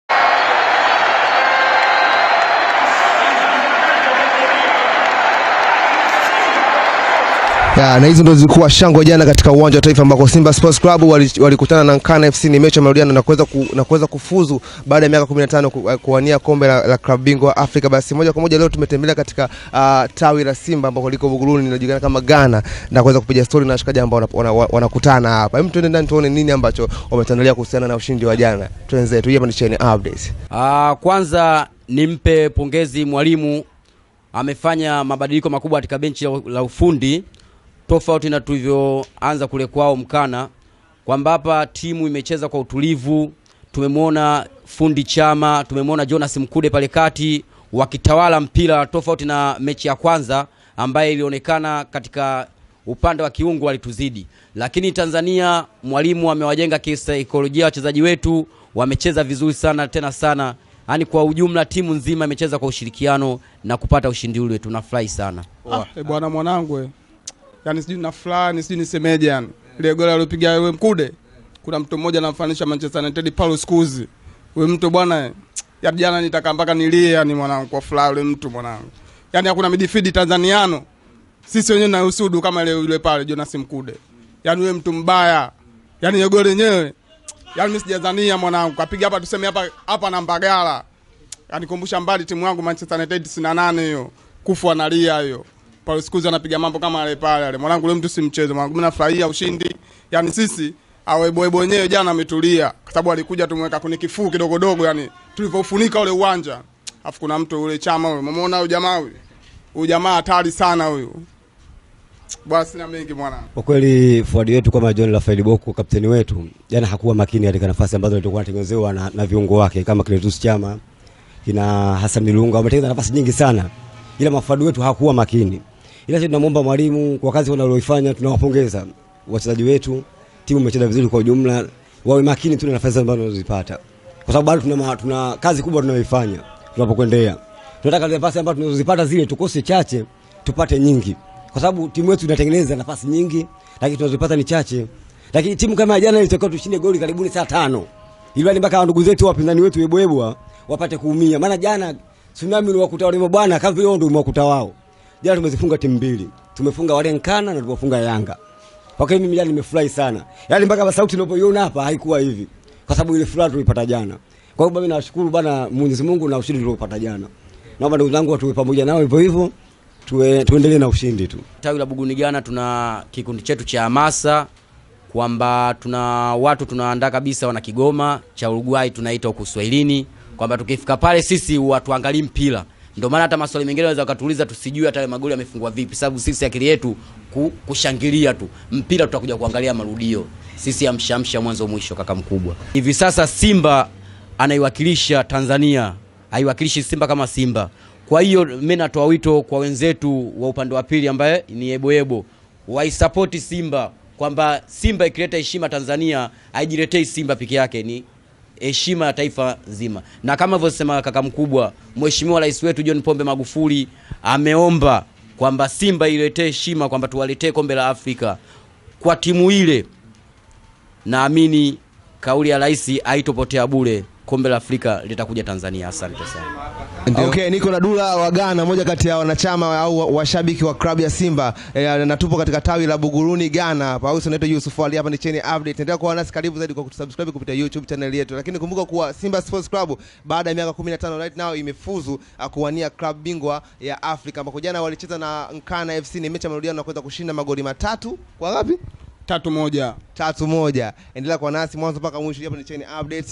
JONAS MKUDE KUFANANISHWA NA KIUNGO WA ZAMANI MAN U.PAUL SCHOLES.
kutokea tawi la simba buguruni ghana..yanga yebo yebo wametulia..